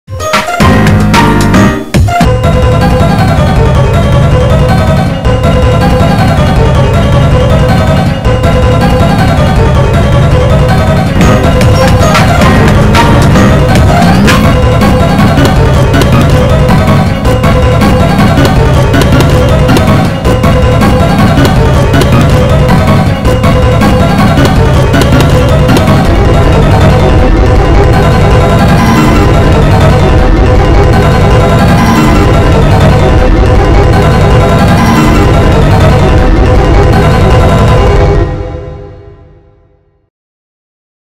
(REUPLOAD) Ward-Csupo Scan (Veg Replace)
Chloe Bourgeois (Queen Bee) dancing it up with the Rose Lavillant (Pigella)